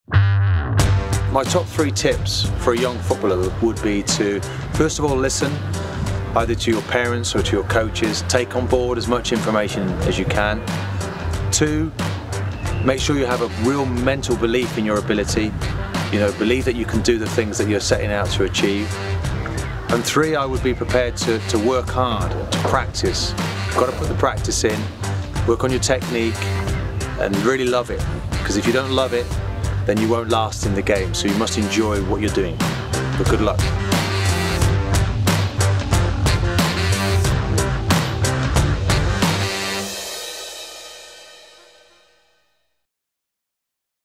Martin Keown DREAM.ON. behind the scenes tips
Working with Wembley FC running up to their 2012/13 FA Cup run Martin Keown gives his tips for success in Football. Watch Wembley FC's next FA Cup game against Uxbridge on Sunday 26th August at 14.30 live streamed to find out if it pays off.